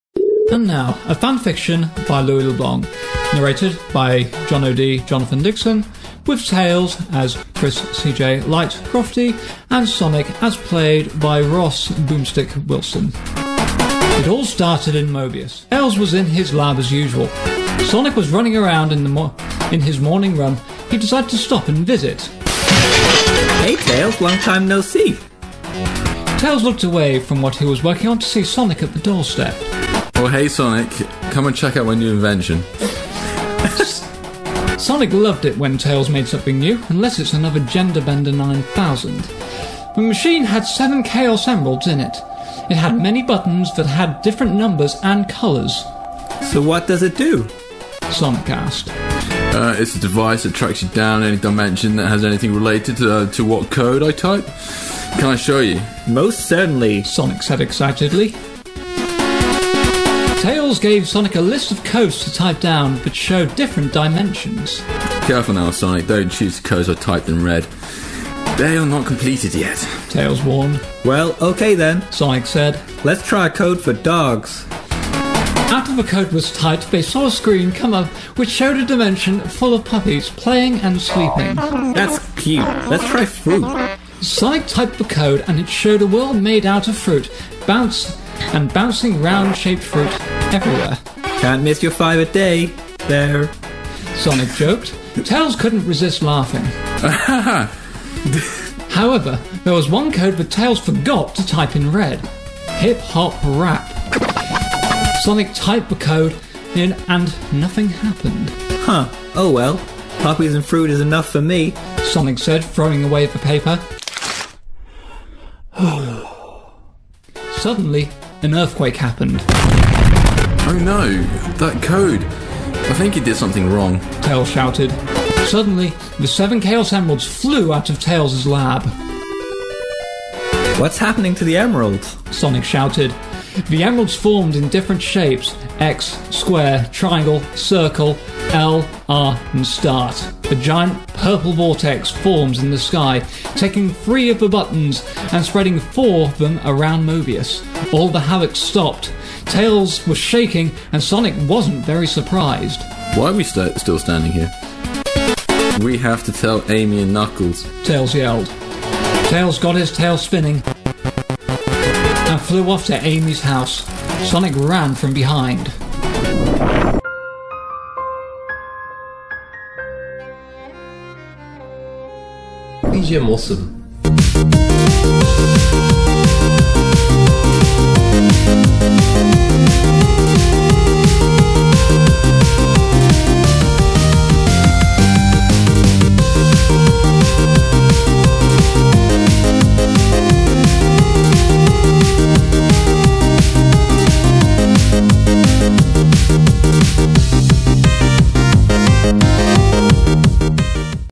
A DRAMATIC fan fiction, DRAMATICALLY animated - The Sonic Show
Remember when our chums at VGM:Awesome did a dramatic read of a submitted Sonic fan fiction... and then Jay animated it (but is too embarrassed credit himself as the master of the visual feast so blamed it on an imaginary intern). well this is that.

► CREDITS:
•The Sonic Show Generation 4.5 Intro Animation: Piggybank of Sonic Paradox
•Original composition for The Sonic Show: Matthew Walker of Red Panda Audio